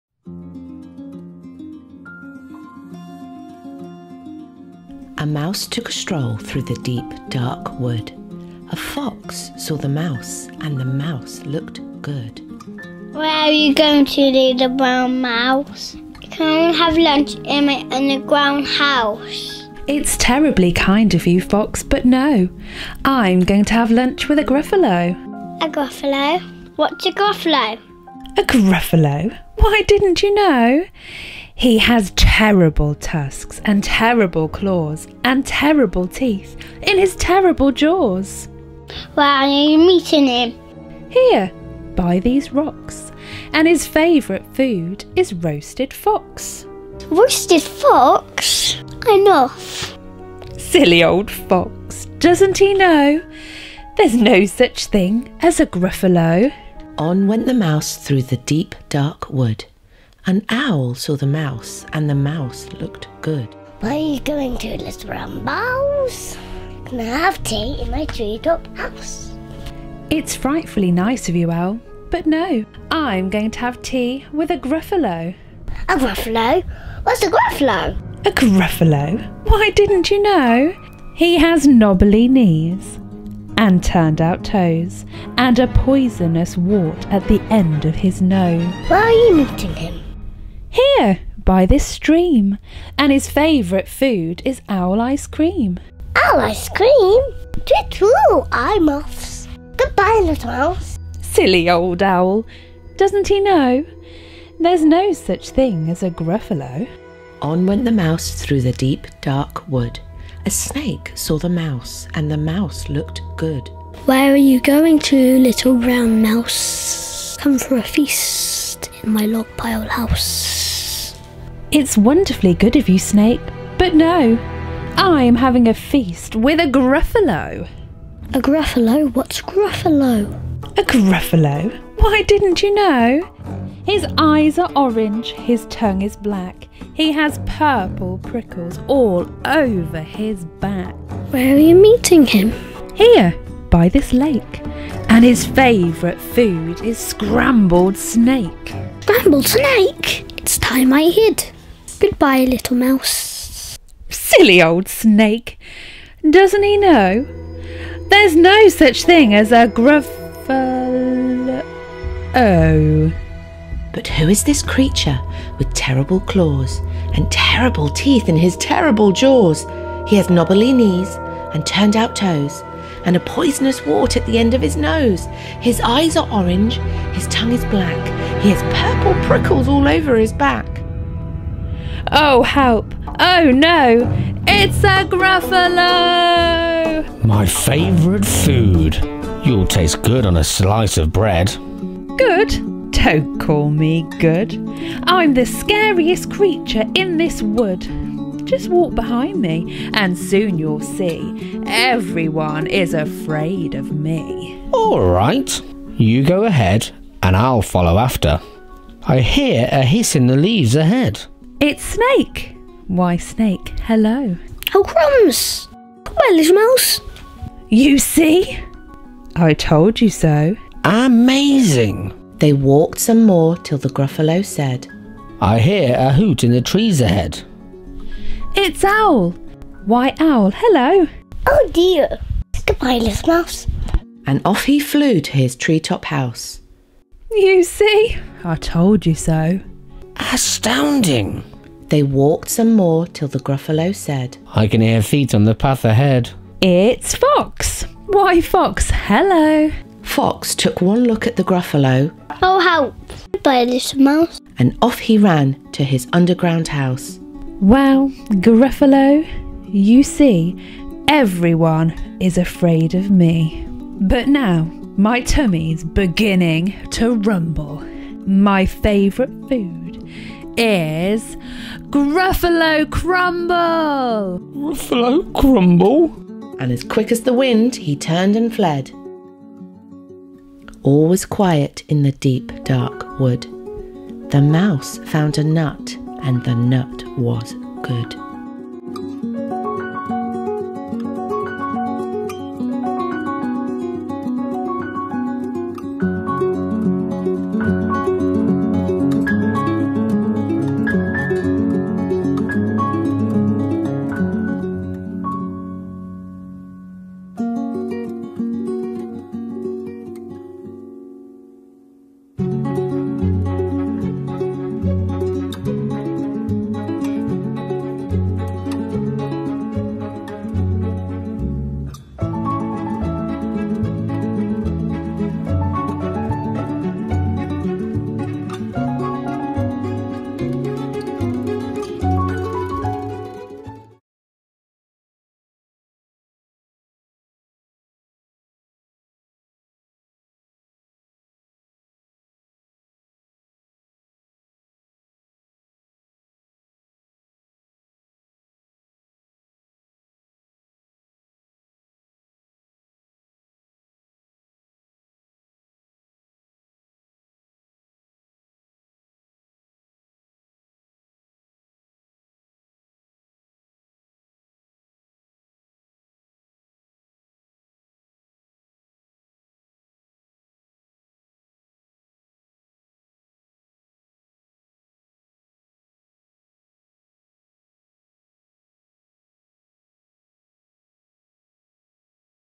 The Gruffalo
"A Gruffalo! Whats a Gruffalo!?"

Come and see our family's take on the wonderful story by Julia Donaldson and illustrated beautifully by Axel Scheffler.

'The Mouse' played by Logan Bowen
Voice by Stacey Bowen
'The Fox' Played and voiced by Theo Bowen
'The Owl' Played and voiced by Wren White
'The Snake' Voiced by Jacob Ward
'The Gruffalo' Voiced by Thomas Bowen
 The Narrator voiced by Katie White